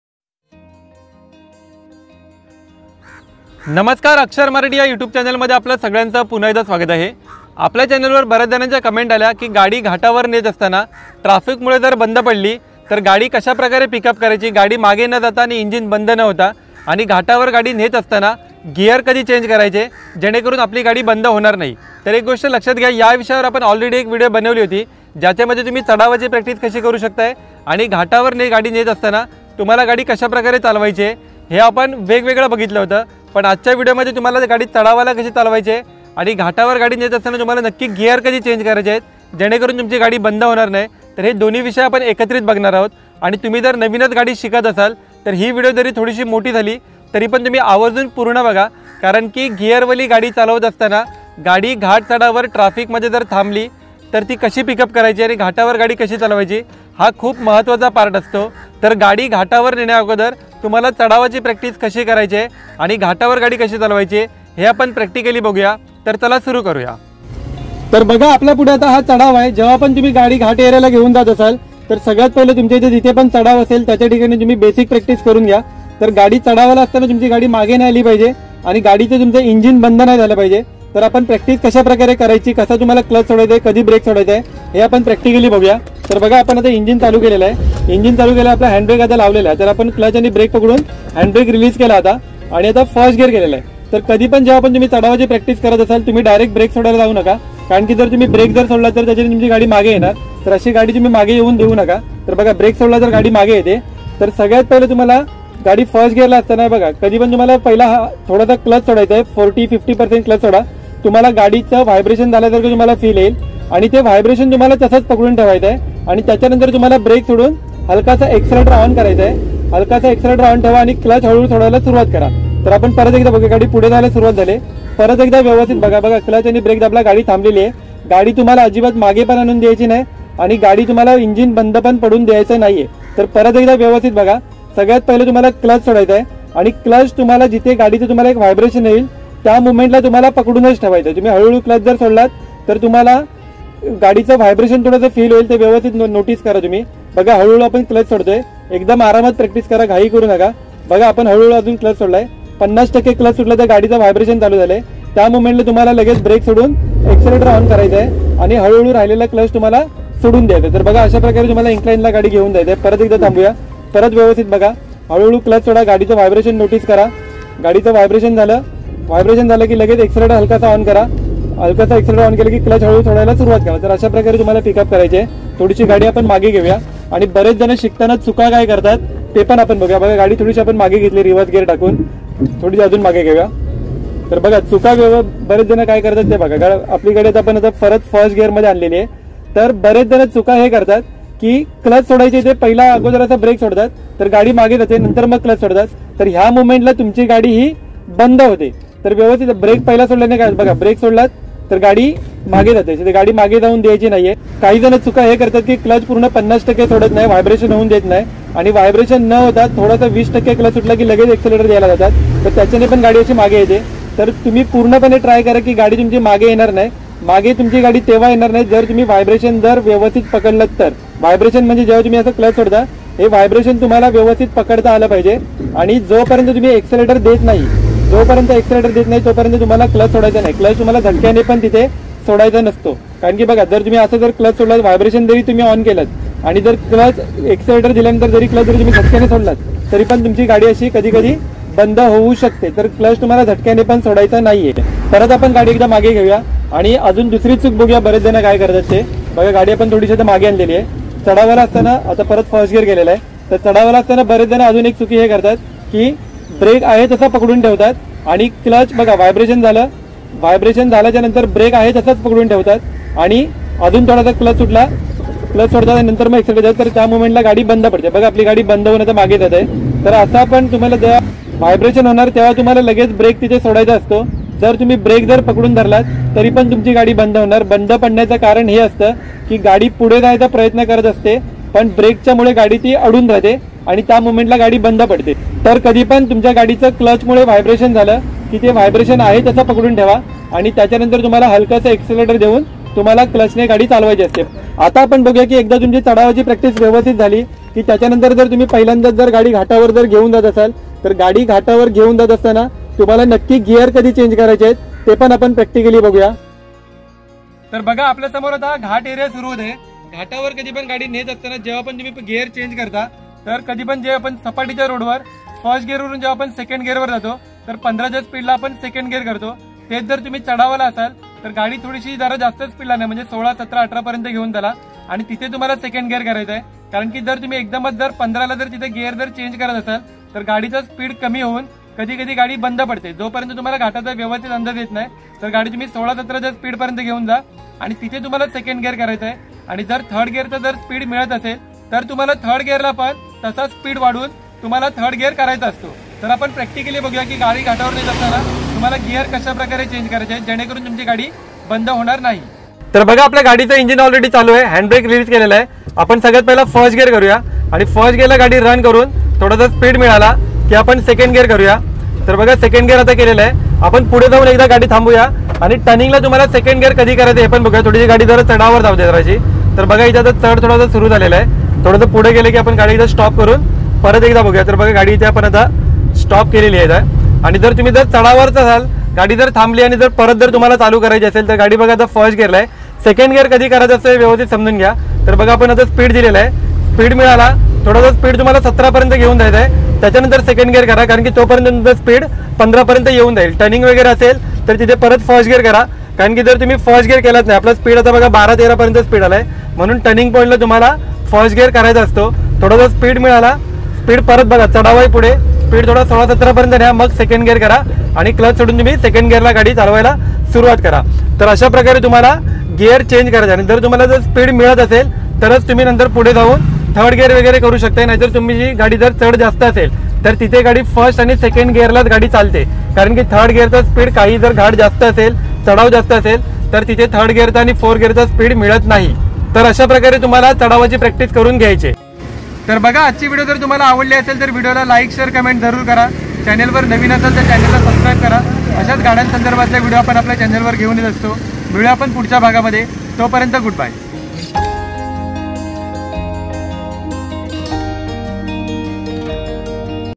६ अ - घाट चढावाला गाडी कशी चालवायची | How to pick a car on incline zone | hill car driving | Marathi
How to drive a car to uphill |